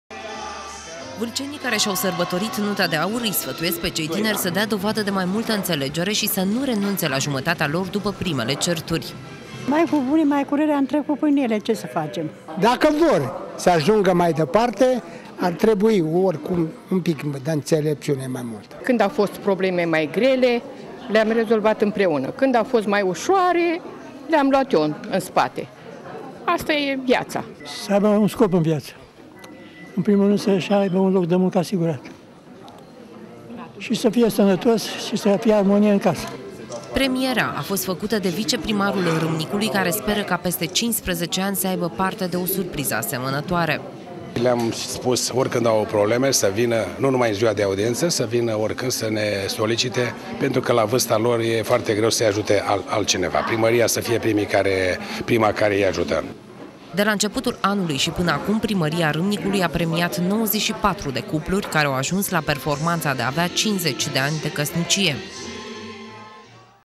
Premiaţi pentru 50 de ani
22 de cupluri din Râmnicu Vâlcea au fost premiate pentru împlinirea a 50 de ani de căsnicie. Oamenii au primit flori, diplome aniversare şi câte un tichet social în valoare de 600 de lei de familie.